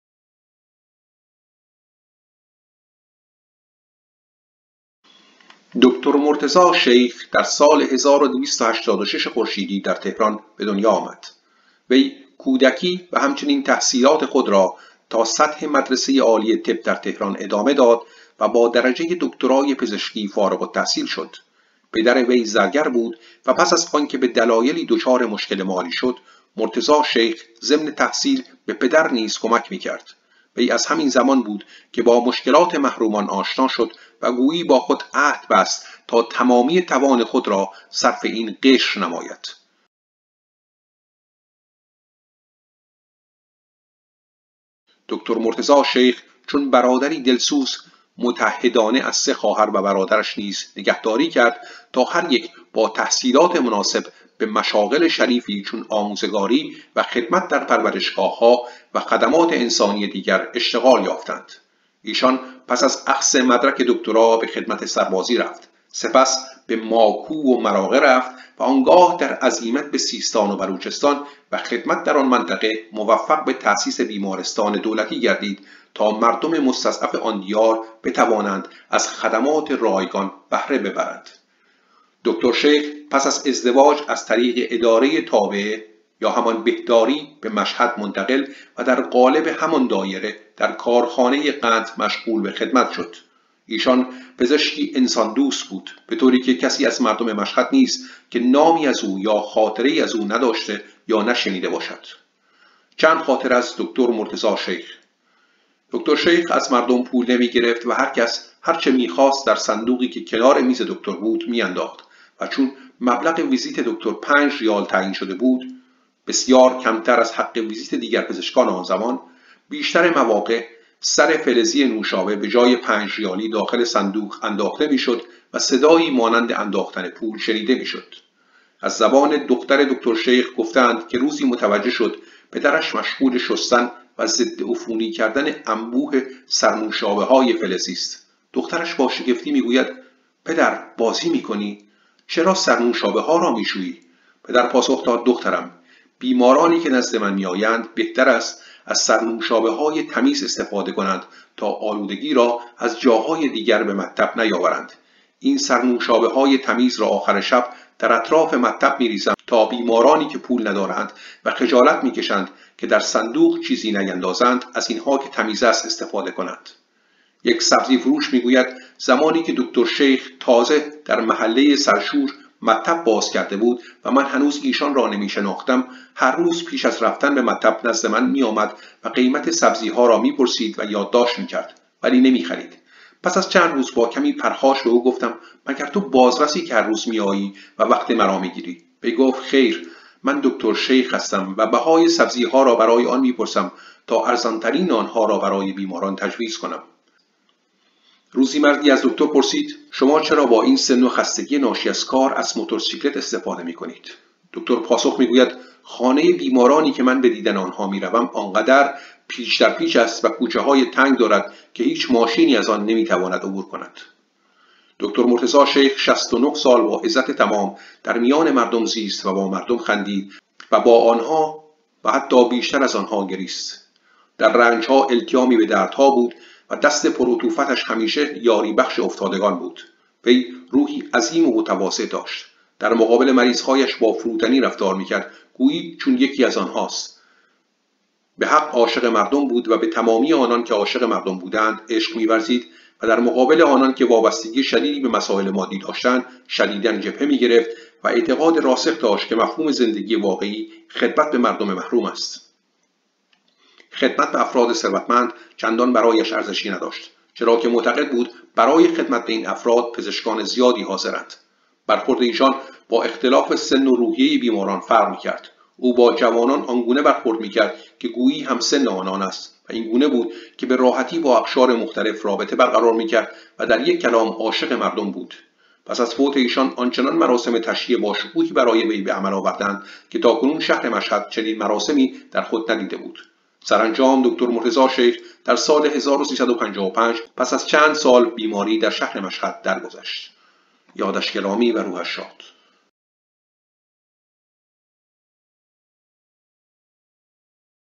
دکتر مرتضی شیخ، پزشکی انساندوست
دکتر مرتضی_شیخ در سال ۱۲۸۶ خورشیدی در تهران به دنیا آمد. وی کودکی و همچنین تحصیلات خود را تا سطح مدرسه عالی طب در تهران ادامه داد و با درجه دکترای پزشکی فارغ التحصیل شد. پدر وی زرگر بود و پس از آنکه به دلایلی دچار مشکل مالی شد، مرتضی شیخ ضمن تحصیل به پدر نیز کمک می‌کرد. وی از همین زمان بود که با مشکلات محرومان آشنا شد و گویی با خود عهد بست تا تمامی توان خود را صرف این قشر نماید. دکتر مرتضی شیخ چون برادری دلسوز، متعهدانه از سه خواهر و برادرش نیز نگهداری کرد تا هر یک با تحصیلات مناسب به مشاغل شریفی چون آموزگاری و خدمت در پرورشگاه‌ها و خدمات انسانی دیگر اشتغال یافتند. وی پس از اخذ مدرک دکترا به خدمت_سربازی رفت. سپس به ماکو و مراغه رفت و آنگاه در عزیمت به سیستان_و_بلوچستان و خدمت در آن منطقه موفق به تأسیس بیمارستان دولتی گردید تا مردم مستضعف آن دیار بتوانند از خدمات رایگان بهره ببرند. دکتر شیخ پس از ازدواج از طریق اداره تابعه (بهداری) به مشهد منتقل و در قالب همان دایره در کارخانه قند مشغول به خدمت شد. وی پزشکی انسان دوست بود، به طوری که کسی از مردم مشهد نیست که نامی از او یا خاطره‌ای از او نداشته یا نشنیده باشد.
چند خاطره از دکتر مرتضی شیخ:
دکتر شیخ از مردم پول نمی‌گرفت و هر کس هرچه می‌خواست در صندوقی که کنار میز دکتر بود می‌ انداخت؛ و چون مبلغ ویزیت دکتر 5 ریال تعیین شده بود (بسیار کمتر از حق ویزیت دیگر پزشکان آن زمان)، بیش‌تر مواقع، سر فلزی نوشابه به جای پنج ریالی داخل صندوق انداخته می شد و صدایی مانند انداختن پول شنیده می‌شد.
از زبان دختر دکتر شیخ گفته‌اند که روزی متوجه شد پدرش مشغول شستن و ضد عفونی کردن انبوه سر نوشابه های فلزی است. دخترش با شگفتی می‌گوید: «پدر بازی می کنی؟ چرا سر نوشابه ها را می شویی؟» پدر پاسخ داد: «دخترم، بیمارانی که نزد من می‌آیند، بهتر است از سر نوشابه‌های تمیز استفاده کنند تا آلودگی را از جاهای دیگر به مطب نیاورند. این سر نوشابه‌های تمیز را آخر شب در اطراف مطب می‌ریزم تا بیمارانی که پول ندارند و خجالت می کشند که در صندوق چیزی نیندازند، از اینها که تمیز است استفاده کنند.»
یک سبزی فروش می‌گوید: زمانی که دکتر شیخ تازه در محله سرشور مطب بازکرده بود و من هنوز ایشان را نمی‌شناختم. هر روز پیش از رفتن به مطب نزد من می‌آمد و قیمت سبزی‌ها را می‌پرسید و یادداشت می‌کرد؛ ولی نمی‌خرید. پس از چند روز، با کمی پرخاش به او گفتم: «مگر تو بازرسی که هر روز می‌آیی و وقت مرا می‌گیری؟» وی گفت: «خیر، من دکتر شیخ هستم؛ و بهای سبزی‌ها را برای آن می‌پرسم تا ارزان‌ترین آنها را برای بیماران تجویز کنم.»
از دکتر حسین خدیوجم نقل است: «روزی در مطب دکتر بودم و او برای بیمارانش آب پاچه تجویز می کرد. از ایشان پرسیدم چرا به جای سوپ جوجه، آب پاچه تجویز می‌کنید؟» وی گفت: «چون برای جبران ضعف بدن بیمار، مانند سوپ جوجه موثر است و مهم‌تر آنکه پاچه گوسفند ارزان است.»
روزی مردی از دکتر می‌پرسد: «شما چرا با این سن و خستگی ناشی از کار، از موتور سیکلت استفاده می‌کنید؟» دکتر پاسخ می‌گوید: «خانه بیمارانی که من به دیدن آنها می‌روم آن قدر پیچ در پیچ است و کوچه‌های تنگ دارد که هیچ ماشینی از آن نمی‌تواند عبور کند.»
دکتر مرتضی شیخ 69 سال با عزت تمام در میان مردم زیست و با مردم خندید و با آنها بیشتر از آنها گریست. در رنجها التیامی به دردها بود و دست پر عطوفتش همیشه یاری بخش افتادگان بود. وی روحی عظیم و متواضع داشت. در مقابل مریض‌هایش با فروتنی رفتار می‌کرد، گویی چون یکی از آنهاست. به حق عاشق مردم بود و به تمامی آنان که عاشق مردم بودند عشق می ورزید و در مقابل آنان که وابستگی شدیدی به مسایل مادی داشتند شدیدا جبهه می‌گرفت و اعتقاد راسخ داشت که مفهوم زندگی واقعی خدمت به مردم محروم است. خدمت به افراد ثروتمند چندان برایش ارزشی نداشت، چرا که معتقد بود برای خدمت به این افراد پزشکان زیادی حاضرند. برخورد ایشان با اختلاف سن و روحیه بیماران فرق می‌کرد. او با جوانان آنگونه برخورد می‌کرد که گویی همسن آنان است و اینگونه بود که به راحتی با اقشار مختلف رابطه برقرار می‌کرد و در یک کلام عاشق مردم بود. پس از فوت ایشان آنچنان مراسم تشییع با شکوهی برای وی به عمل آوردند که تا کنون شهر مشهد چنین مراسمی در خود ندیده بود. سرانجام دکتر مرتضی شیخ در سال 1355 پس از چند سال بیماری در شهر مشهد درگذشت.